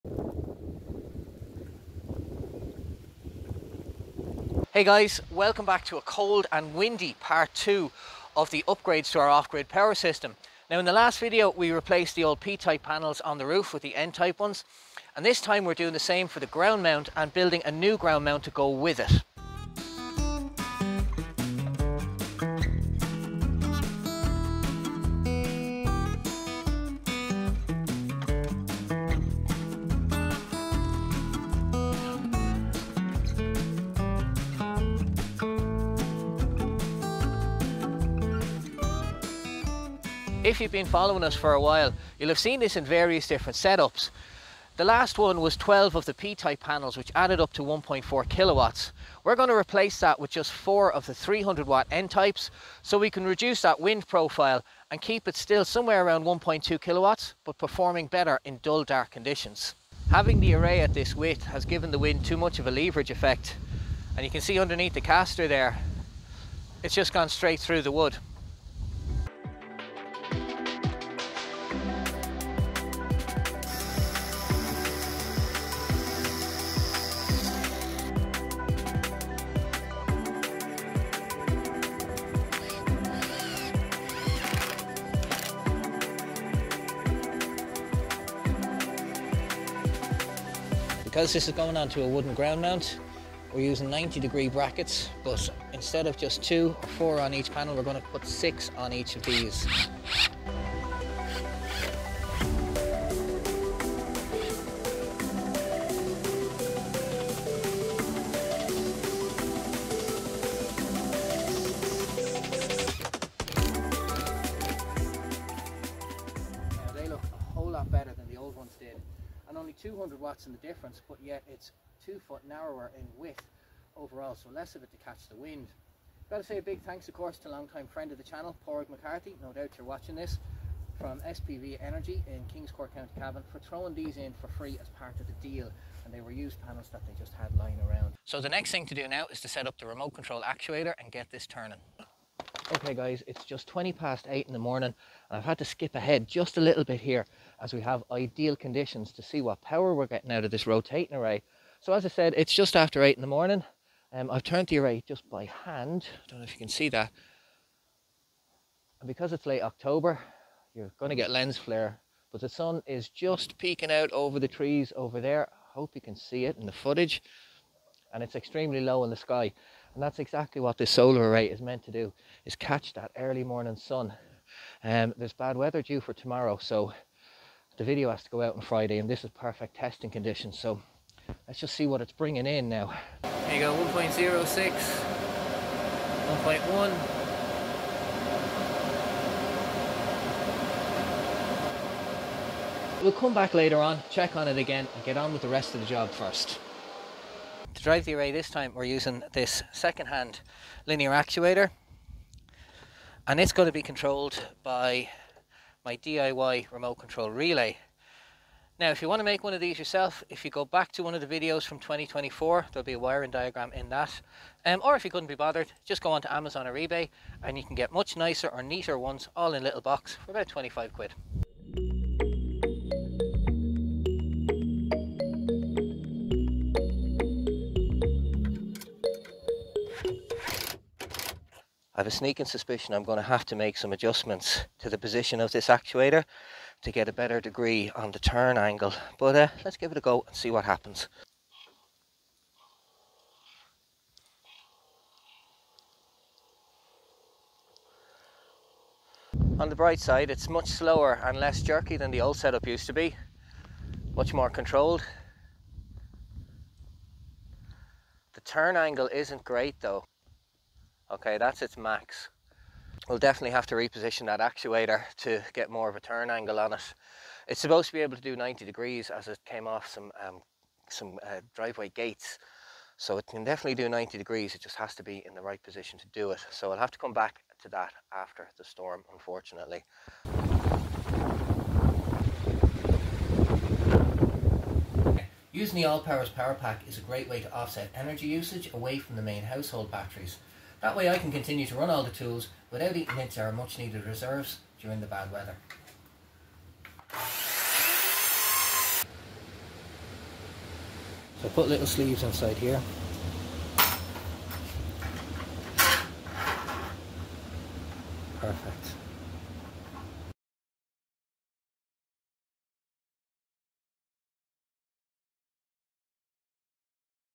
The Rotating Solar Array, Off Grid Upgrades pt2
The Ground Mounts. Repairing and Upgrading the Off Grid Rotating Solar Array. Replacing the old P-type panels with newer more efficient N-type for better performance in dull conditions and more stability in strong winds.